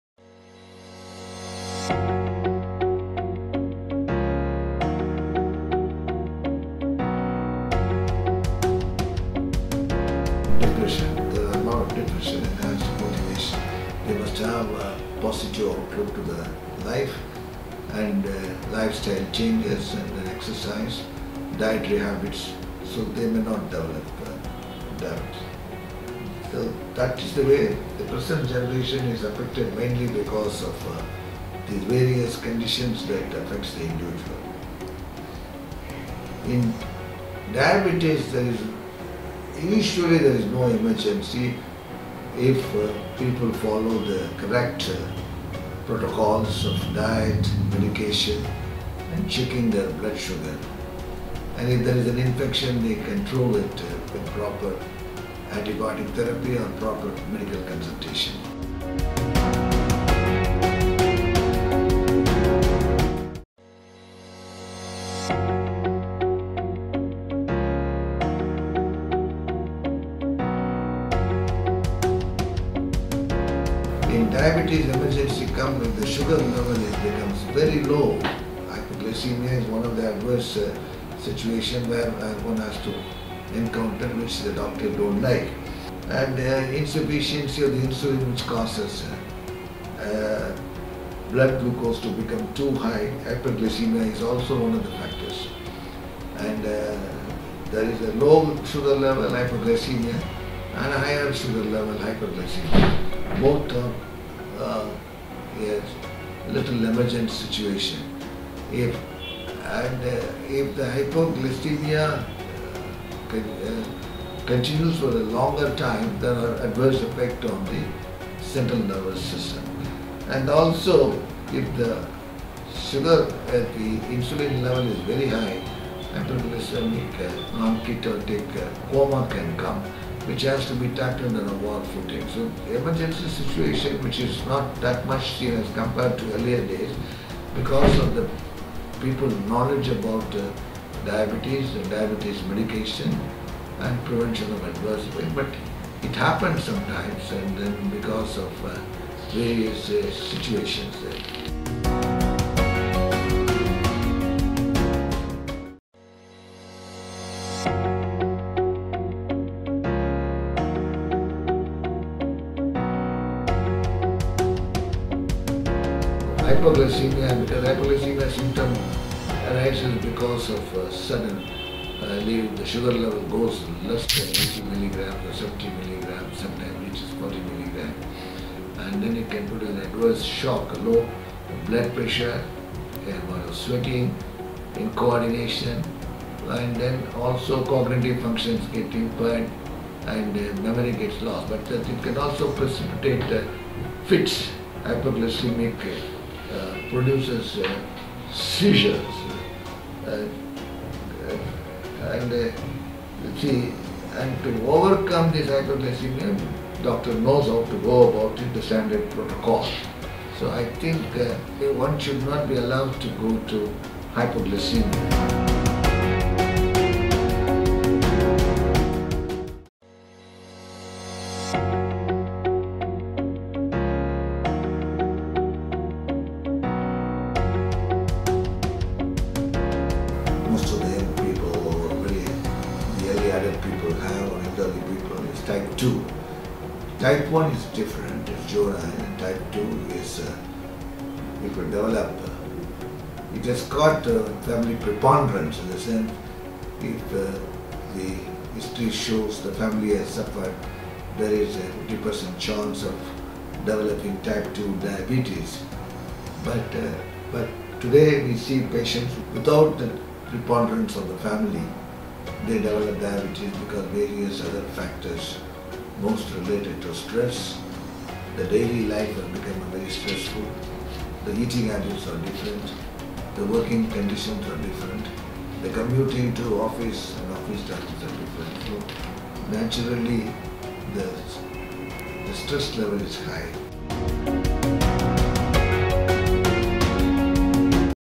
Dr. N. Rajagopal
Diabetes is one of the most dangerous and rapidly growing diseases in India. Dr. N. Rajagopal, a leading Diabetologist explains about the following aspects of Diabetes in this video. Please watch and share!
Importance of lifestyle modification in diabetes
. 0:00
What is a Diabetic Emergency?
1:04
Is type 2 diabetes hereditary?
3:40